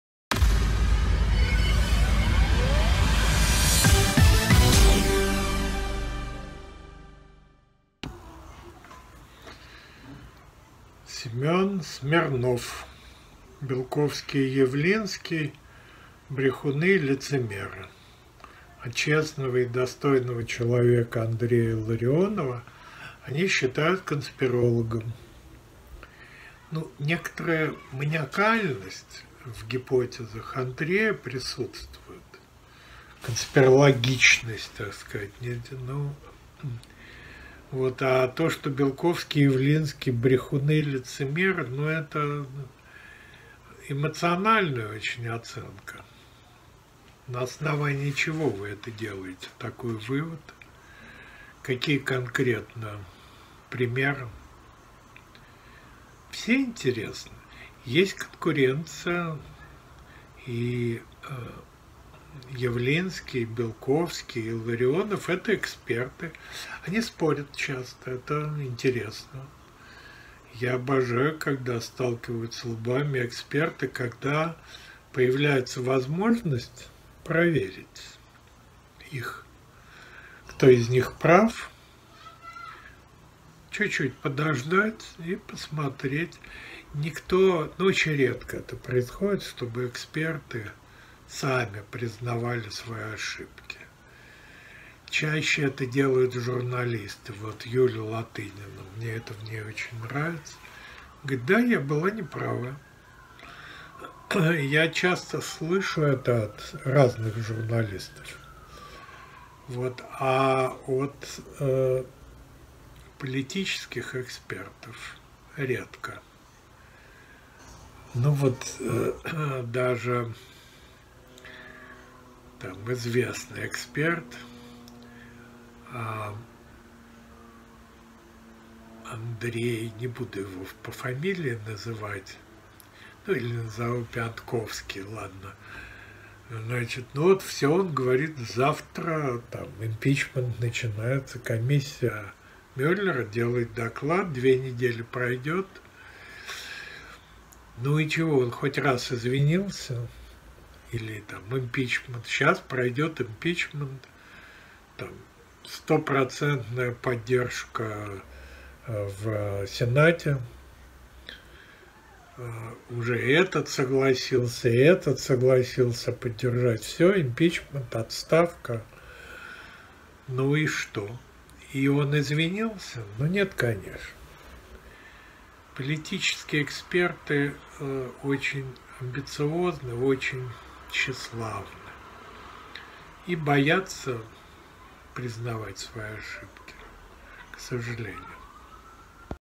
Илларионов - конспиролог
Очередная книга известного российского предпринимателя и политика, бывшего главного редактора журнала «Америка» Константина Борового описывает события 1999 года, когда за два года до теракта 11 сентября 2001 года в Нью-Йорке он получил информацию о подготовке этого теракта и передал ее посольству США в Москве, а затем и руководству ФБР в США. Автор рассказывает о вовлеченности российских спецслужб в деятельность против США и Запада до 2000 года и особенно после прихода советских чекистов во власть в России вместе с Путиным. Большая часть книги описывает период «перезагрузки» или, благодаря ошибке переводчиков «перегрузке», когда беспечное руководство США считало возможным договориться с советскими чекистами о мирном сосуществовании. Как мы теперь знаем, безуспешно. О том, как успешно ФСБ руками американских спецслужб действовало против противников Путина. Как мы теперь знаем, продолжает действовать.